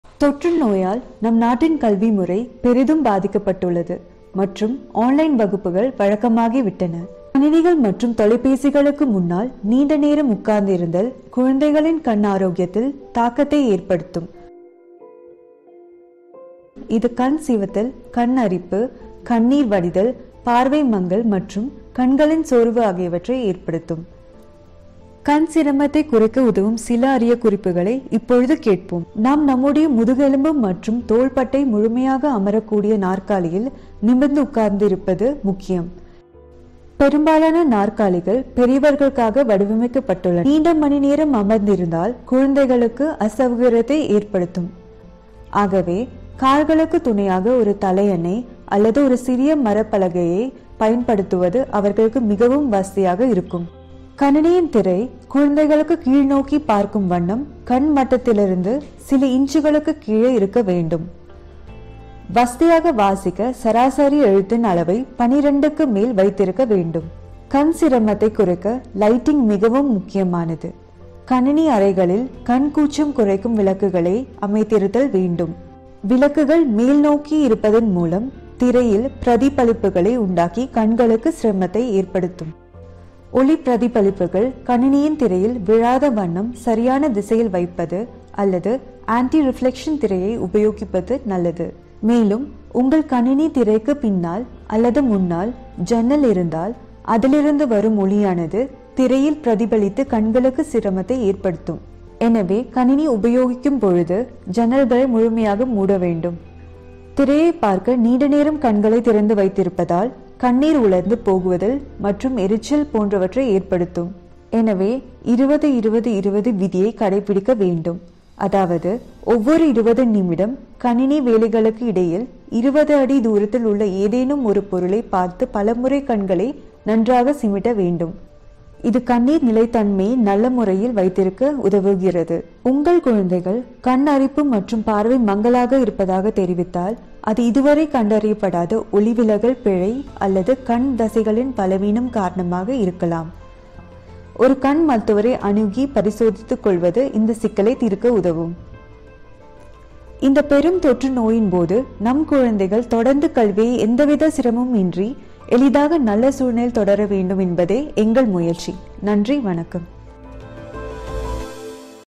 ஆன்லைன் வகுப்புகளின் போது குழந்தைகள் கடைபிடிக்க வேண்டிய கண் பாதுகாப்பு வழிமுறைகள்
Keeping an EYE on your children at home during online classes